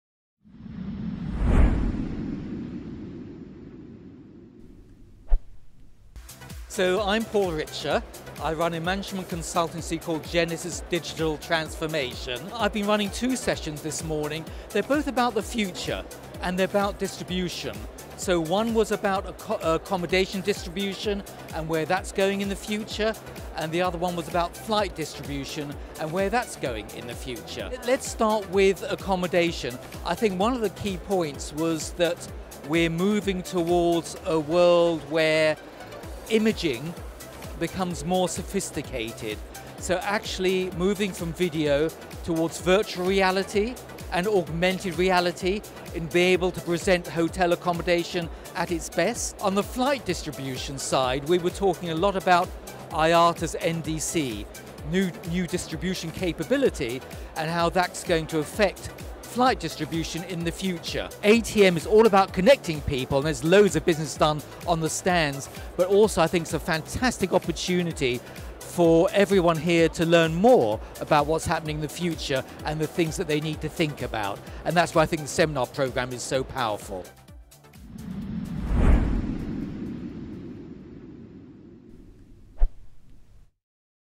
Distribution set to change dramatically for flights and accommodation...
Distribution set to change dramatically for flights and accommodation according to Paul Richer of Genesys Digital Transformation